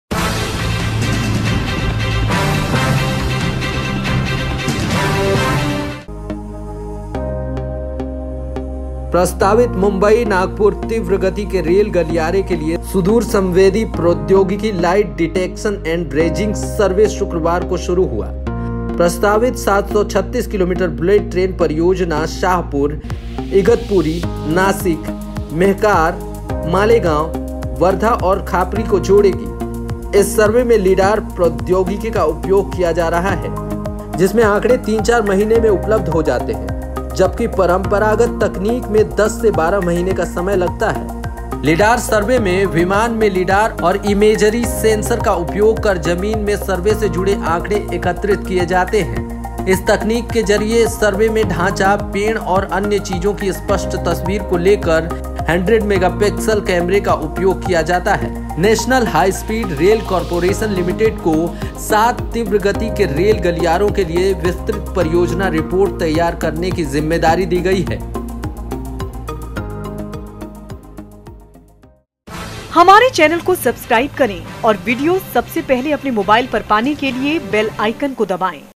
Mumbai - Nagpur Bullet Train Project | मुंबई-नागपुर के बीच प्रस्तावित बुलेट ट्रेन के लिए काम शुरू
प्रस्तावित मुंबई-नागपुर तीव्र गति के रेल गलियारे के लिए सुदूर संवेदी प्रौद्योगिकी लाइट डिटेक्शन एंड रेंजिंग (लीडार) सर्वे शुक्रवार को शुरू हुआ। प्रस्तावित 736 किलोमीटर बुलेट ट्रेन परियोजना शाहपुर, इगतपुरी, नासिक, मेहकार, मालेगांव, वर्धा और खापरी को जोड़ेगी।

इस सर्वे में लीडार प्रौद्योगिकी का उपयोग किया जा रहा है जिसमें आंकड़े 3-4 महीने में उपलब्ध हो जाते हैं जबकि परंपरागत तकनीक में 10 से 12 महीने का समय लगता है।

लीडार सर्वे में विमान में लीडार और इमेजरी सेंसर का उपयोग कर जमीन के सर्वे से जुड़े आंकड़े एकत्रित किये जाते हैं। इस तकनीक के जरिये सर्वे में ढांचा, पेड़ और अन्य चीजों की स्पष्ट तस्वीर को लेकर 100 मेगापिक्सल कैमरे का उपयोग किया जाता है। एनएचएसआरसीएल (नेशनल हाईस्पीड रेल कॉरपोरेशन लि.) को सात तीव्र गति के रेल गलियारों के लिये विस्तृत परियोजना रिपोर्ट तैयार करने की जिम्मेदारी दी गयी है।

The Remote Sensing Technology Light Detection and Ranging (LiDAR) survey for the proposed Mumbai-Nagpur high speed rail corridor began on Friday. The proposed 736-km bullet train project will connect Shahpur, Igatpuri, Nashik, Mehkar, Malegaon, Wardha and Khapri.

LiDAR technology is being used in this survey in which data becomes available in 3-4 months whereas traditional technique takes 10 to 12 months.